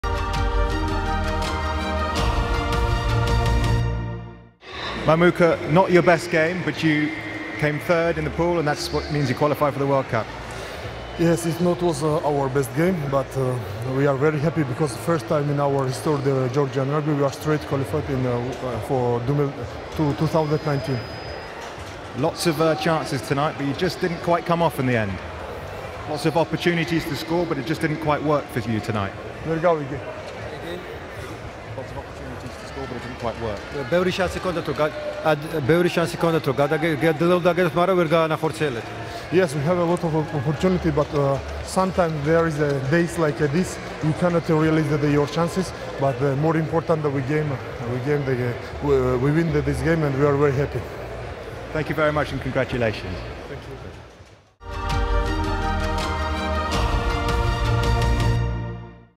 Gorgodze very happy with narrow victory for Georgia
Georgia climbed above Tonga in the race for third place in Pool C but only after an heroic - if not always legal - rearguard action by Namibia, who broke their Rugby World Cup points duck at Sandy Park on Wednesday night.

Barely credibly, the Africans survived the loss of their captain, Jacques Burger, to a concussion injury after 10 minutes, as well as two late yellow cards yet still managed to lead 6-0 at half-time.

The ferocity of the Namibians’ defence was matched only by the profligacy of the Georgians’ error-strewn handling as two first-half touchdowns were ruled out, for a knock-on and a forward pass.

Logic suggested it couldn’t last and eventually Georgia’s man-mountain of a captain, Mamuka Gorgodze (pictured above), powered over for a try in the 50th minute.

THIRD YELLOW CARD

Merab Kvirikashvili converted but only after number eight Renaldo Bothma became the third Namibian to be shown a yellow card, this time for a high tackle in the build-up to the try.

Flagging visibly, Namibia could not halt the tide and, five minutes later, a Georgian lineout led to fly-half Lasha Malaguradze bursting over for a second try and a 14-6 lead.